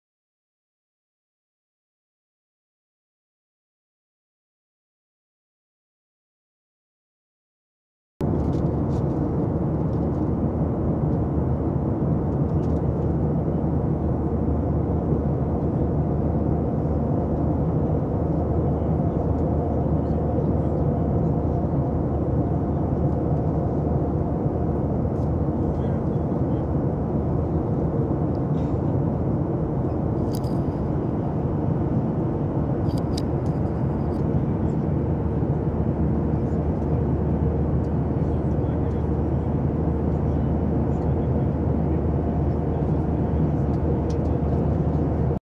Встречный самолёт над Европой Мальта 2019
Во время полёта заснял самолёт.